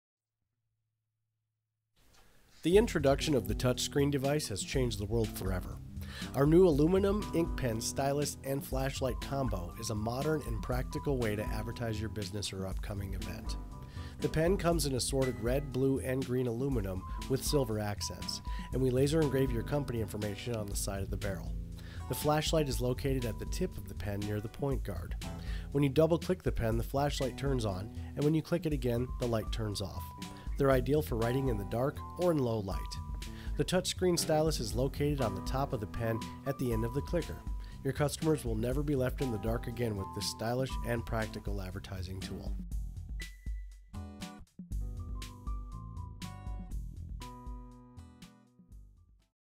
Graphco Line: The AMSL
Introducing the AMSL. Aluminum pen with stylus and light.